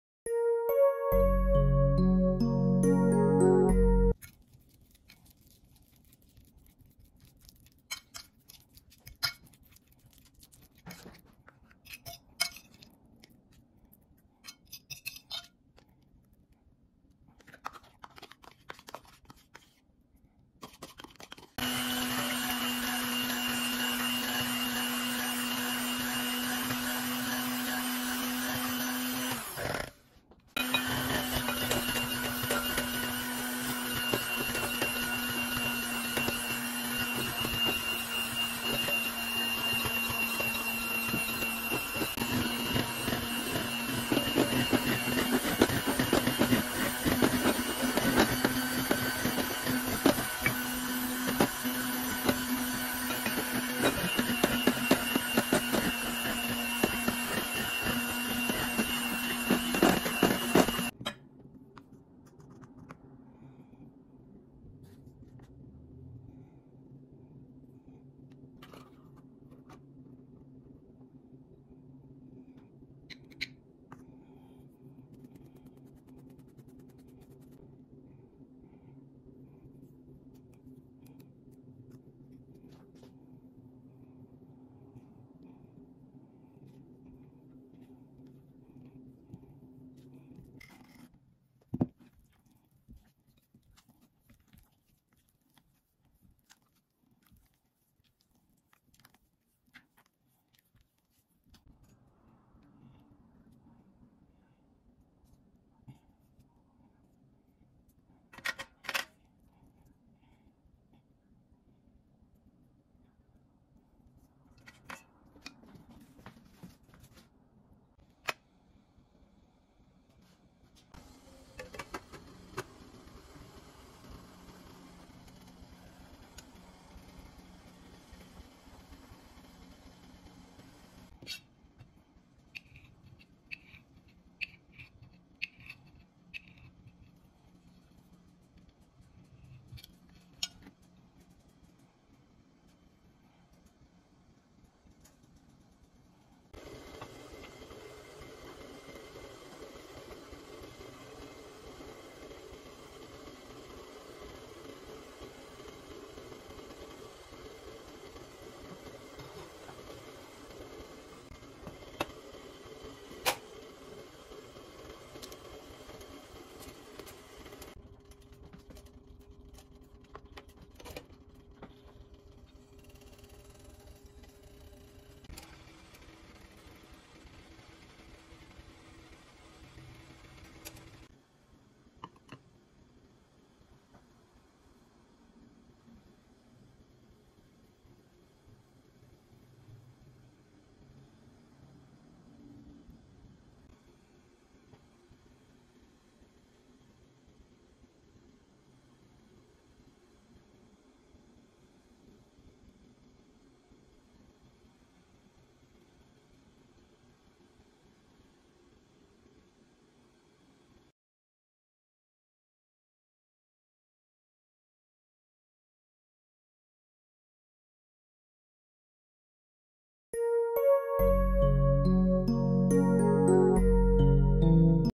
Huwag Itapon ang Over Ripe Banana. Gawin Nyo Ito! Pwede Pang-Negosyo! Banana Puto Muffin!
Pwede ito pang-meryenda, dessert, o pambaon ng inyong mga anak!